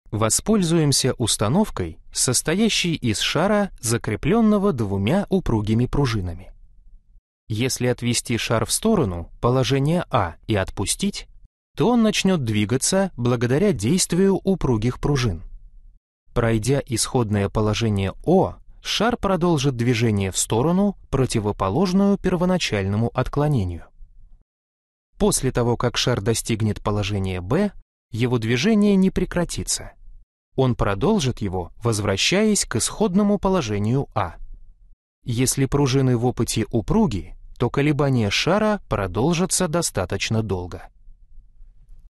Демонстрация закона сохранения на примере пружинного маятника
За учебником Физика 7 класс Перышкин А. В.

Воспользуемся установкой, состоящей из шара, закреплённого между двумя упругими пружинами
Если отвести шар в сторону — положение А — и отпустить, то он начнёт двигаться благодаря действию упругих пружин.
Пройдя исходное положение О, шар продолжит движение в сторону, противоположную первоначальному отклонению.
После того как шар достигнет положения В, его движение не прекратится. Он продолжит его, возвращаясь к исходному положению О и далее к положению А.
Если пренебречь силой трения в системе тел шар — пружина, то колебания будут продолжаться бесконечно.
Если пружины в опыте упруги, то колебания шара продолжаться достаточно долго.